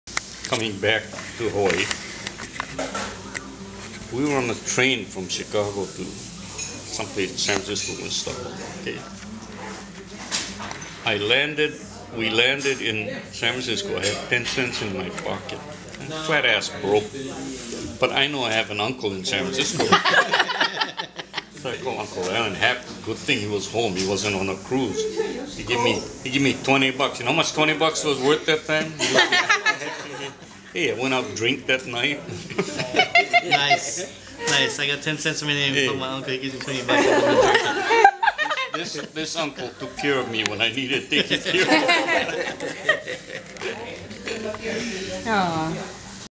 Shigi saves Harris's broke ass
This is the story my mom's cousin Harris tells about my great uncle Shigi (aka Uncle Alan) from when he was a kid.

"...coming back to hawaii we were on the train from chicago to someplace in San Francisco.  We stop, okay... I landed, we landed in San Francisco.  I had ten cents in my pocket.  Flat-ass broke!

But I know I have an uncle in San Francisco.

So I call Uncle Alan.  Good thing he was home; he wasn't on a cruise.

He gave me 20 bucks.  You know how much 20 bucks is worth that time?  Hey, I went out and drink that night.

This uncle took care of me when I needed taking care of."